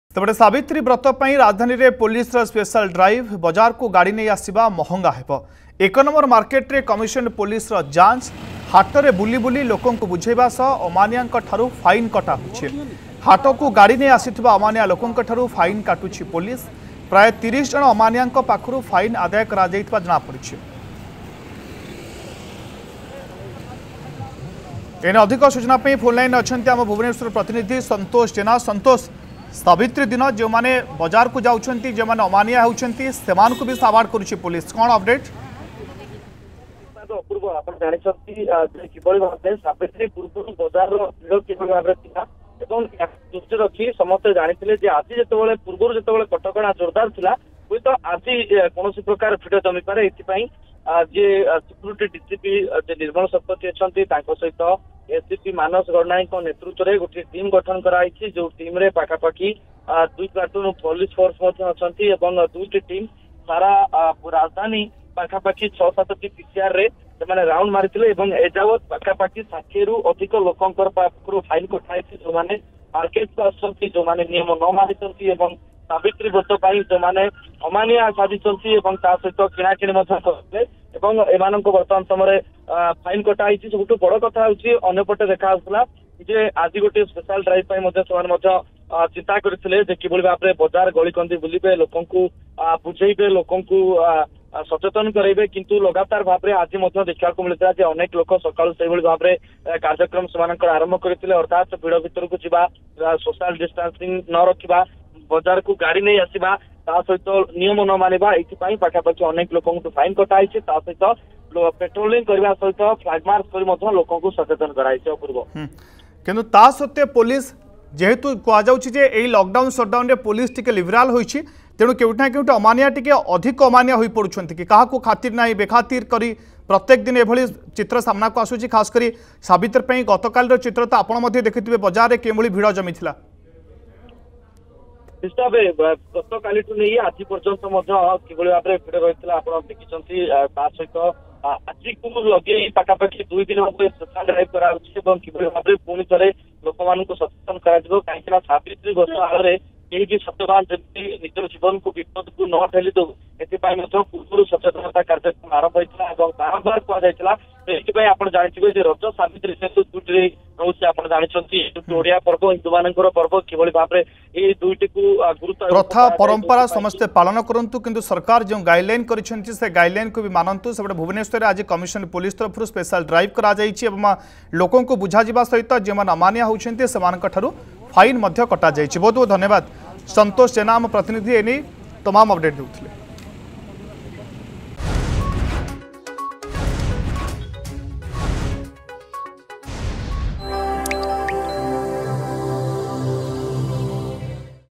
Savitri Brata: Police Drive In Bhubaneswar (ସାବିତ୍ରୀ ବ୍ରତ ପାଇଁ ରାଜଧାନୀରେ ପୋଲିସର ସ୍ପେଶାଲ ଡ୍ରାଇଭ)
ସାବିତ୍ରୀ ବ୍ରତ ପାଇଁ ରାଜଧାନୀରେ ପୋଲିସର ସ୍ପେଶାଲ ଡ୍ରାଇଭ । ବଜାରକୁ ଗାଡି ନେଇ ଆସିବା ହେବ ମହଙ୍ଗା । ୧ ନଂ ମାର୍କେଟରେ କମିସନରେଟ୍ ପୋଲିସର ଯାଞ୍ଚ ।